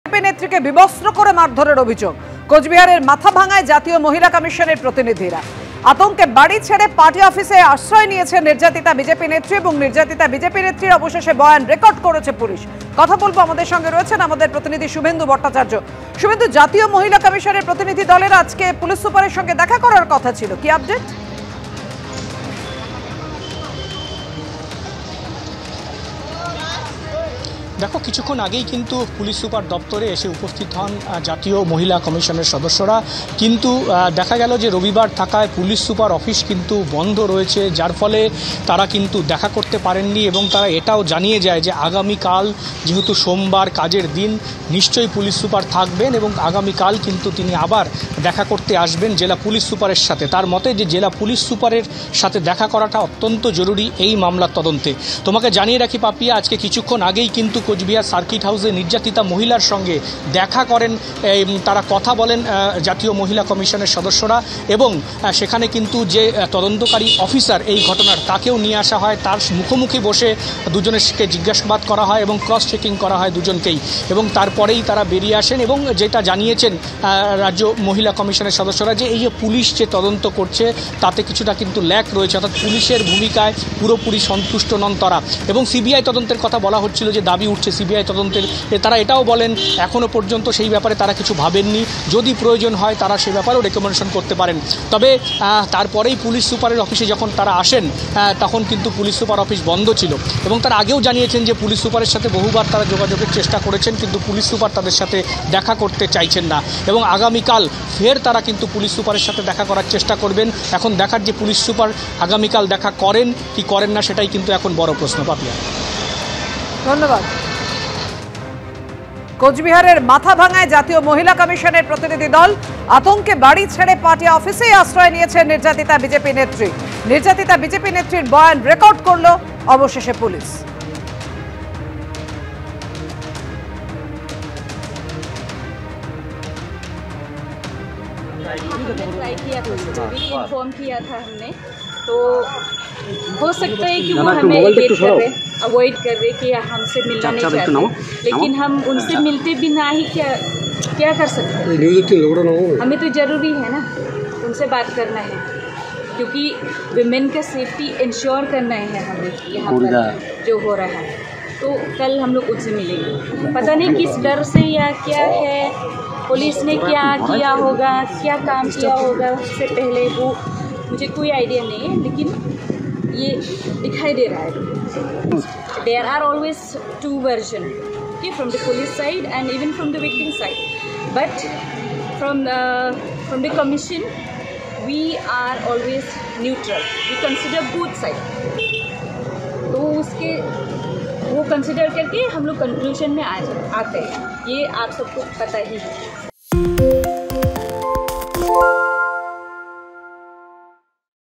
Coochbehar: বিজেপি নেত্রীকে বিবস্ত্র করে মারধরের অভিযোগ, মাথাভাঙায় জাতীয় মহিলা কমিশনের প্রতিনিধিরা
West Bengal news: কোচবিহারে (Coochbehar) বিজেপি নেত্রীকে বিবস্ত্র করে মারধরের অভিযোগ। কোচবিহারের মাথাভাঙায় জাতীয় মহিলা কমিশনের প্রতিনিধি। আতঙ্কে বাড়ি ছেড়ে পার্টি অফিসেই আশ্রয় নির্যাতিতা বিজেপি নেত্রীর। নির্যাতিতা বিজেপি নেত্রীর বয়ান রেকর্ড পুলিশের।